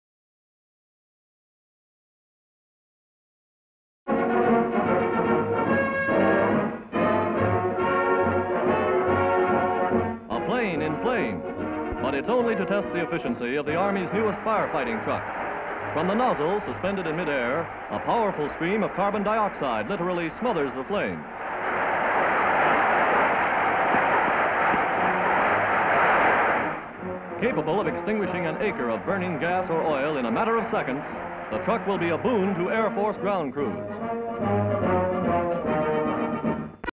Army Air Force truck extinguishes fire by carbon dioxide in United States. HD Stock Footage
Army Air Force truck extinguishes fire by carbon dioxide in United States.

New fire fighter demonstrated by Army Air Force. Tests conducted by Air Force fire truck that extinguishes fire by carbon dioxide. Long pipe on truck throws carbon dioxide on fire and extinguishes it. Location: United States. Date:  1944.

57,000+ broadcast-quality historic clips for immediate download.